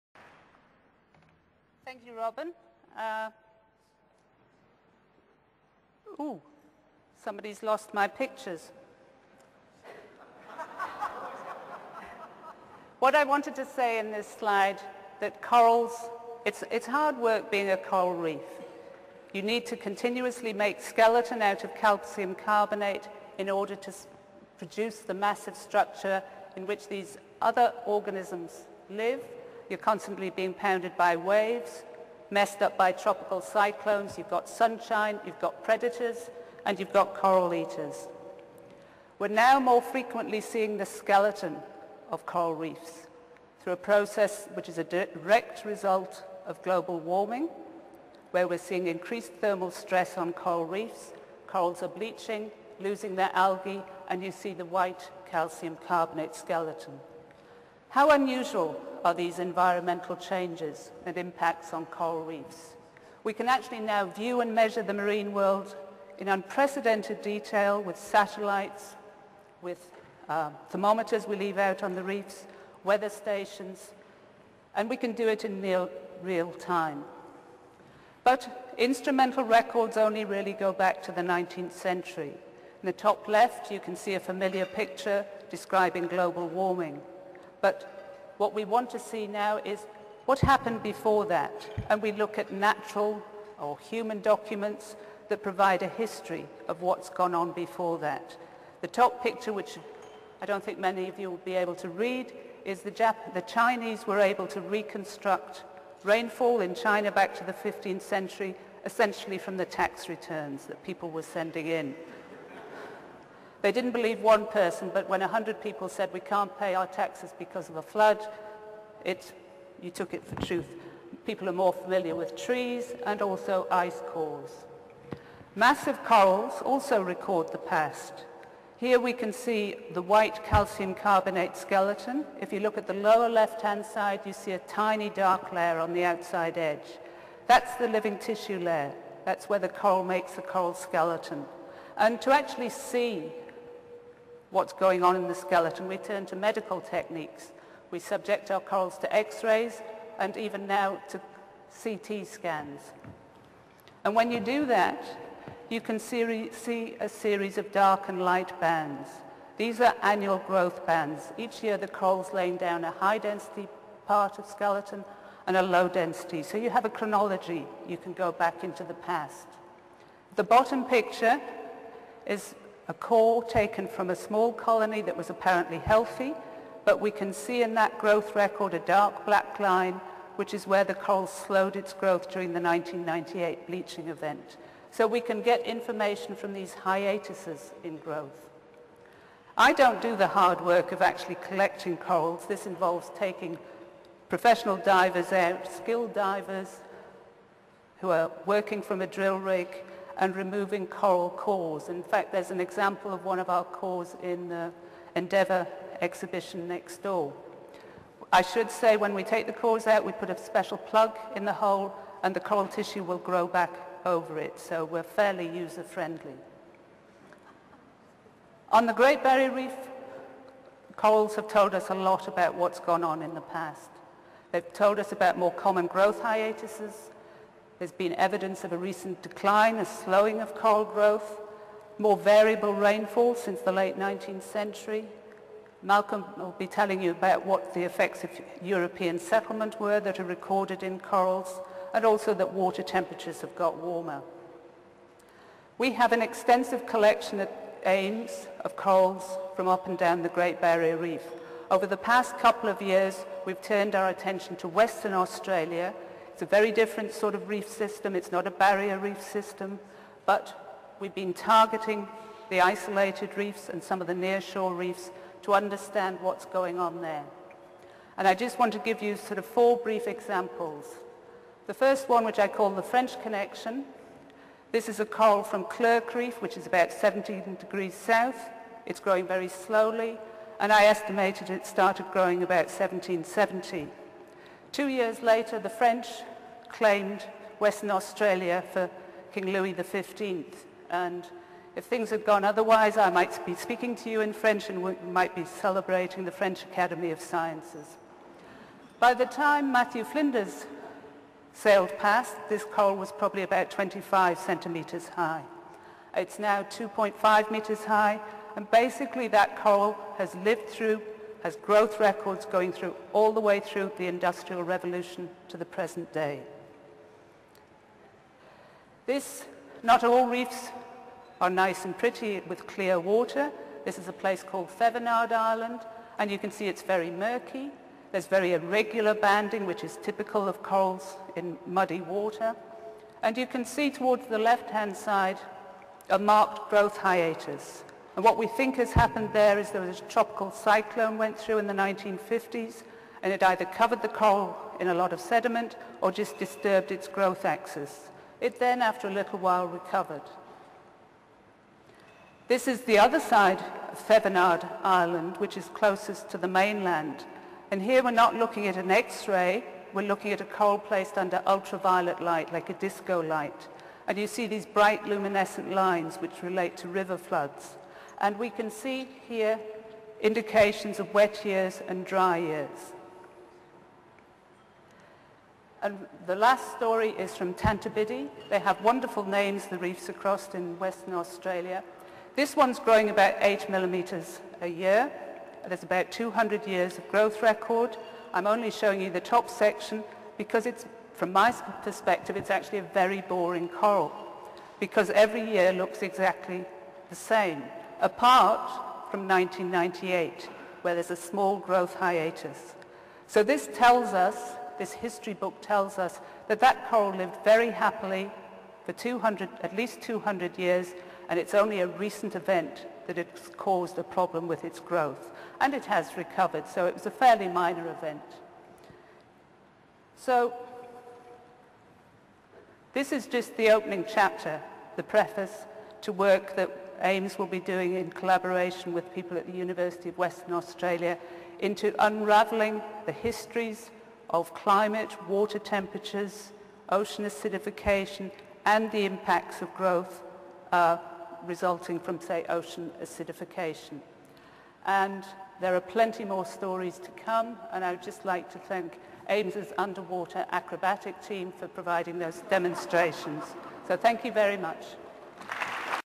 Janice Lough - Coral history books: new chapters from Western Australian reefs
Seminar title: Coral history books: new chapters from Western Australian reefs
Presented by: Janice Lough
Seminar type: Public forum

Presentation given at the ARC Centre of Excellence for Coral Reef Studies 2010 public forum: Coral Reefs in a Changing Environment.

Abstract:
Coral reefs contain their own history books in annually banded massive coral skeletons.  Cores from such corals provide perspectives on past coral reef climates and environmental impacts - beyond the range of instrumental observations.  New WA coral reef cores are now sharing their secrets.

Bio:
Dr Janice Lough is a Senior Principal Research Scientist leading the Responding to Climate Change Team at the Australian Institute of Marine Science and a Partner Investigator with the ARC Centre of Excellence for Reef Studies. Trained as a climatologist at the Climate Research Unit (UK), her research interests focus on understanding the nature, causes and impacts of climate variability and climate change in tropical marine environments. She also specializes in obtaining historical perspectives on coral reefs and the significance of currently observed changes using the rich archive of proxy environmental information contained in long-lived massive coral skeletons.